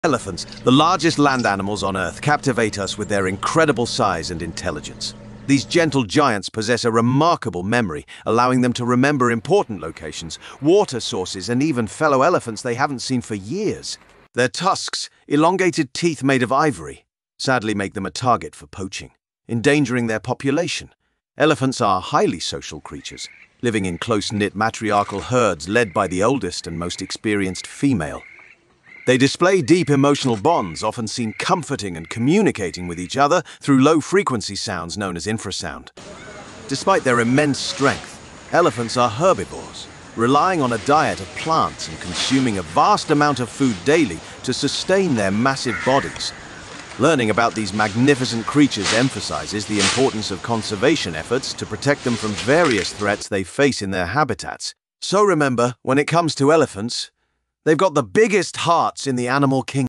Animal Facts - Elephants
Elephants, the largest land animals on Earth, captivate us with their incredible size and intelligence. These gentle giants possess a remarkable memory, allowing them to remember important locations, water sources, and even fellow elephants they haven't seen for years. Their tusks, elongated teeth made of ivory, sadly make them a target for poaching, endangering their population. Elephants are highly social creatures, living in close-knit matriarchal herds led by the oldest and most experienced female. They display deep emotional bonds, often seen comforting and communicating with each other through low-frequency sounds known as infrasound. Interestingly, elephants have a strong sense of empathy and mourn their deceased, showcasing the depth of their emotional intelligence. Despite their immense strength, elephants are herbivores, relying on a diet of plants and consuming a vast amount of food daily to sustain their massive bodies. Learning about these magnificent creatures emphasizes the importance of conservation efforts to protect them from various threats they face in their habitats. So remember, when it comes to elephants, they've got the "biggest" hearts in the animal kingdom!